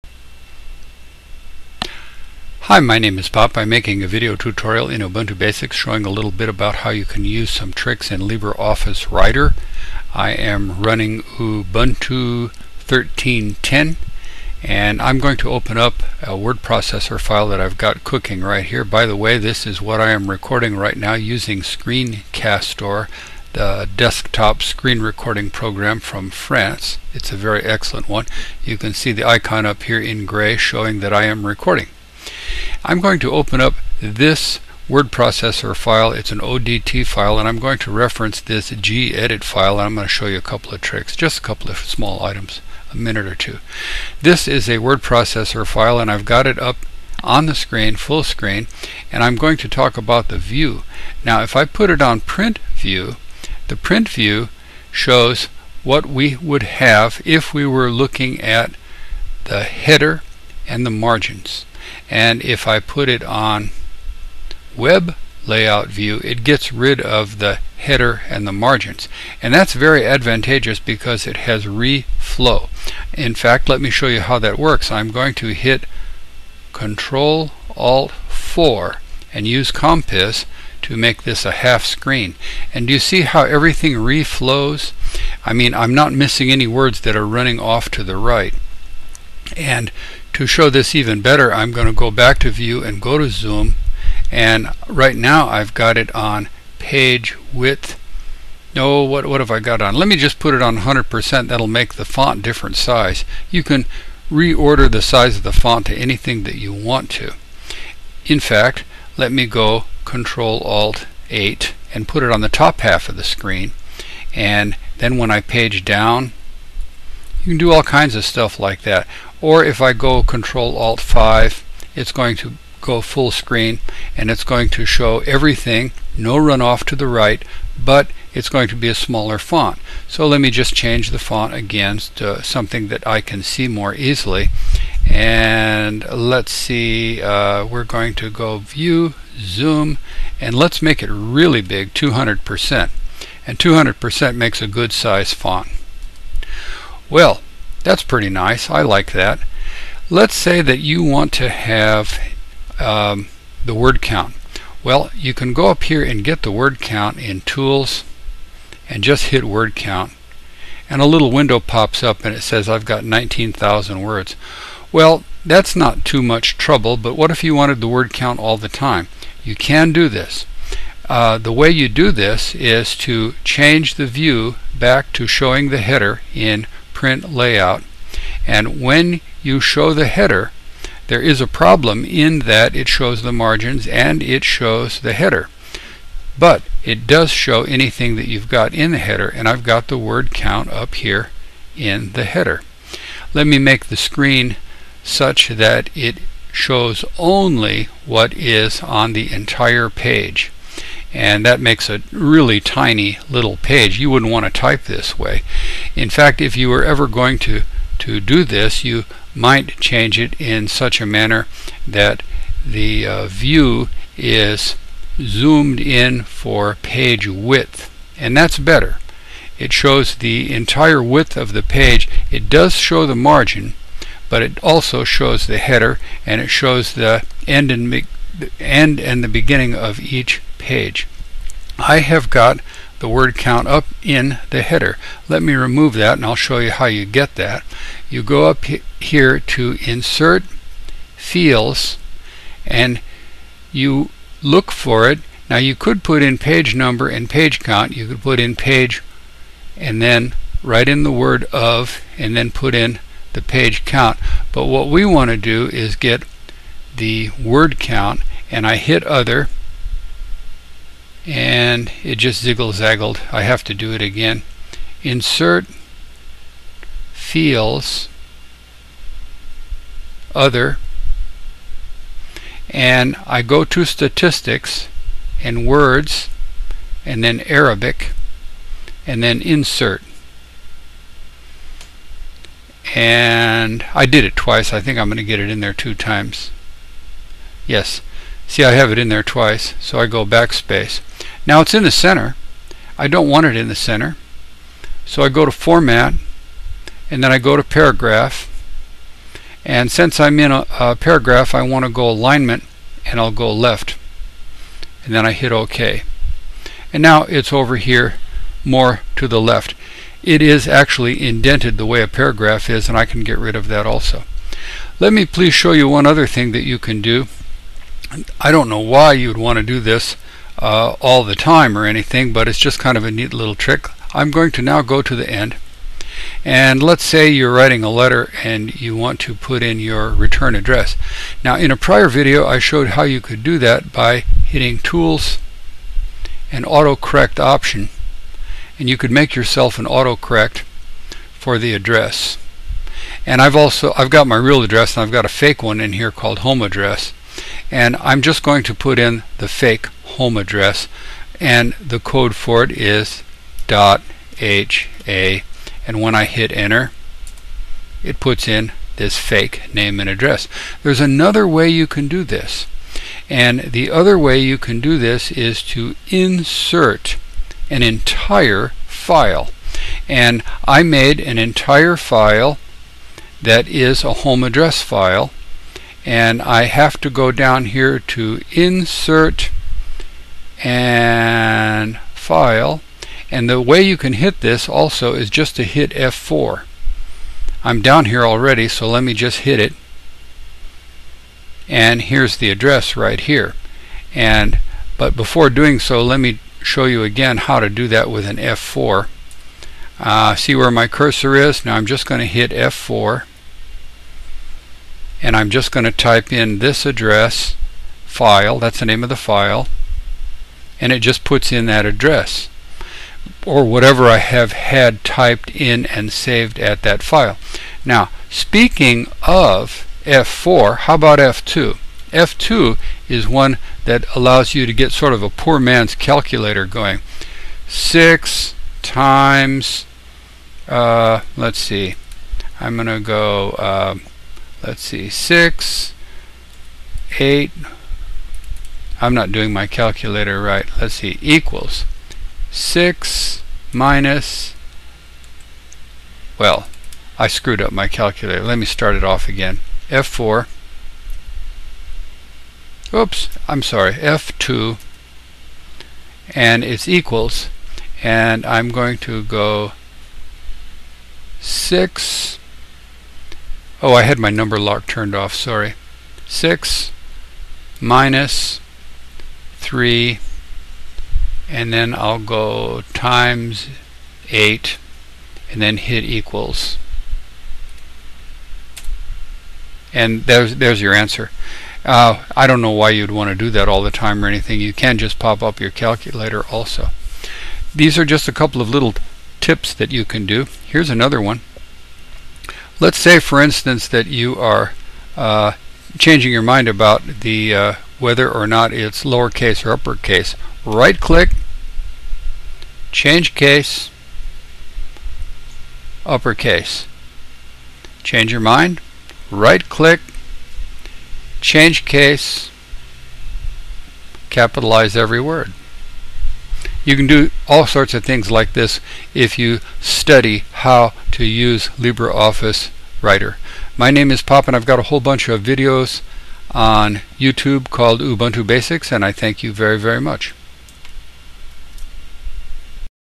Writer Tips & Tricks
Libreoffice Writer offers thousands of time-saving features, but you have to dig them out.  You can insert "boiler plate" text with F4.  F4 inserts an entire file at your cursor. (In a prior video I showed more-or-less the same thing via another method:  using 'dot commands' in Auto Correct)  You can inert a live word count or live page count into your header.  You can use web-view to enable auto flow for any convenient font size or any convenient window size.  Web view not only has auto flow, but it also temporarily erases margins and headers from view.  Print view does show exactly how the page appears when printed, but it requires you to use a small font -- otherwise the text will run off the page to the right.  F2 lets you pop up a poor man's calculator.  The answer automatically inserts at the cursor.  You can right click while on any word and change the case.  If you highlight an entire sentence or paragraph you can do the same for any selected text.